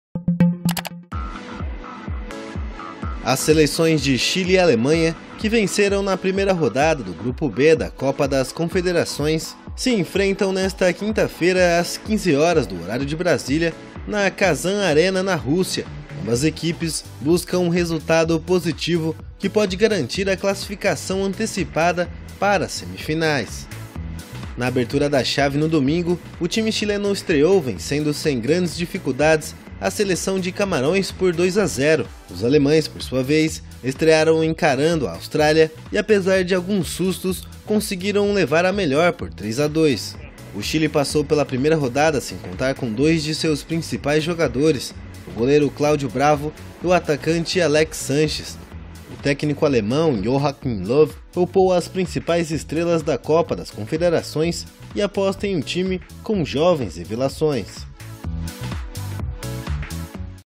Chile e Alemanha fazem duelo de líderes no grupo B da Copa das Confederações
Kazan (Rússia), 21 jun (EFE/EPA).=. As seleções de Chile e Alemanha, que venceram na primeira rodada do grupo B da Copa das Confederações, se enfrentarão nesta quinta-feira, às 15h (de Brasília), na Kazan Arena, de olho em novo resultado positivo, que pode até assegurar classificação antecipada às semifinais.

Domingo, na abertura da chave, em Moscou, a 'Roja' estreou passando por Camarões por 2 a 0, sem grande dificuldades. Um dia depois, os campeões mundiais estrearam encarando a Austrália, tomaram alguns sustos, em Sochi, mas conseguiram levar a melhor por 3 a 2. EFE TV
wdc/ imagens: Maxim Shipenkov e Maxim Shipenkov (EFE/EPA). Palabras clave: copa das confederacoes,alemanha,chile,russia,epa,efe